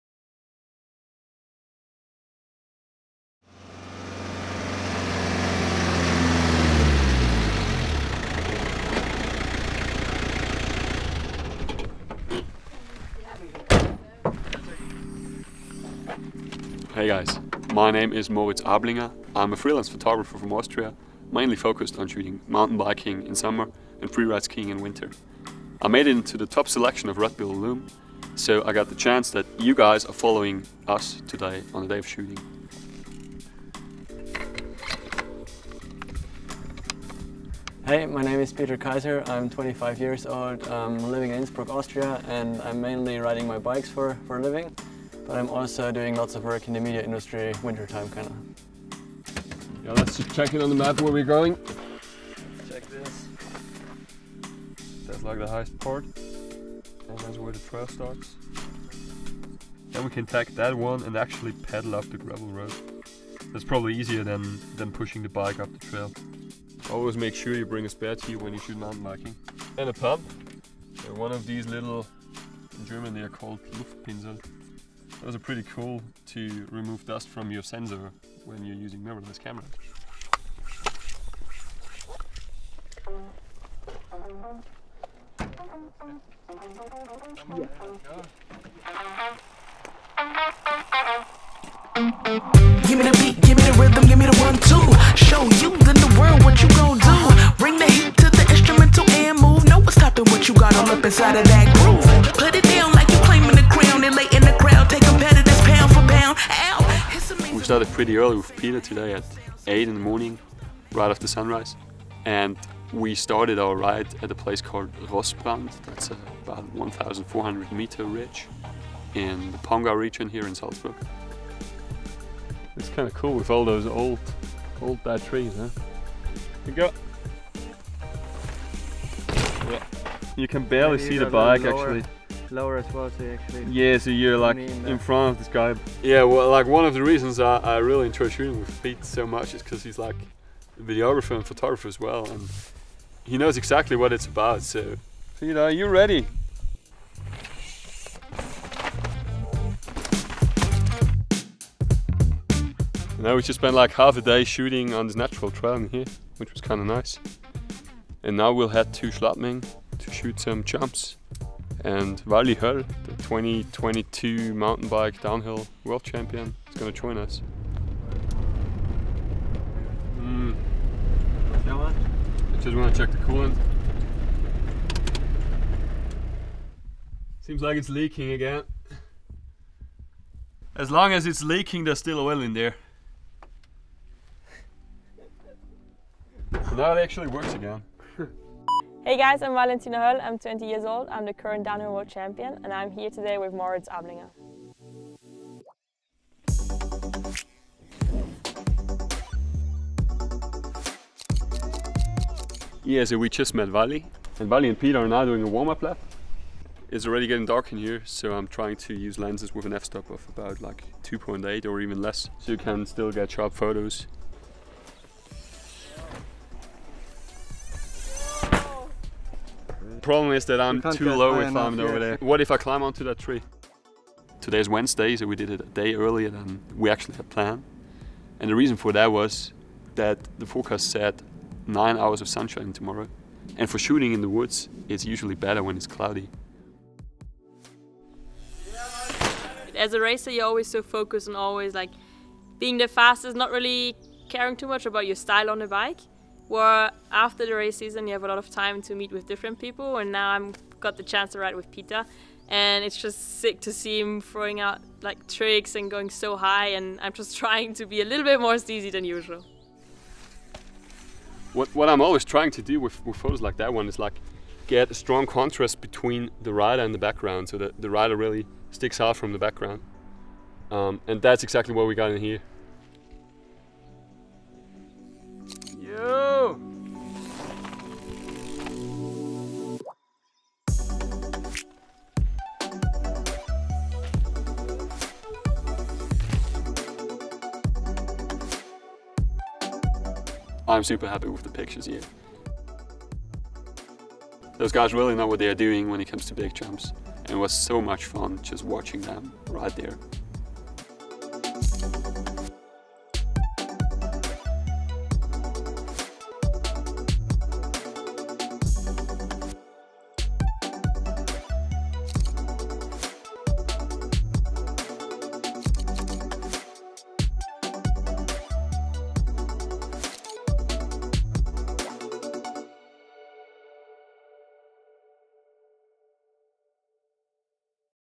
MOUNTAIN BIKE PHOTOGRAPHY WITH MORITZ ABLINGER (ft., VALI HÖLL and PETER KAISER)
We head into the woods with the Red Bull Illume semi-finalist Moritz Ablinger to see how he shoots pro-riders.

Flat light is usually the photographer’s worst enemy and photographers spend hours looking at weather forecasts to find those moments when the light is at its best to bring color and contrast to a shot. But when you’re shooting mountain bikers in a forest, the opposite is true.

This is one of the advice Moritz gives as he takes us behind the scenes on a mountain bike shoot with pro athletes  @valihoell  and @PeterKaiser26   in two locations in the Austrian mountains – Nature Trail Rossbrand and Bikepark Schladming.

Together the athletes and photographer were able to work on the images they had in mind, then analyze the results using the @Lenovo   Yoga 9i Gen 7 (14”) flagship convertible laptop.